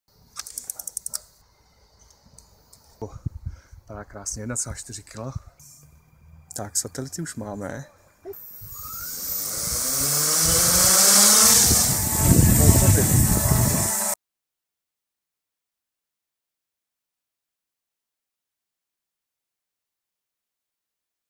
Xu Gong Maiden
Jakub Steiner (@jimmacfx) showing and maiden flying his Xu Gong.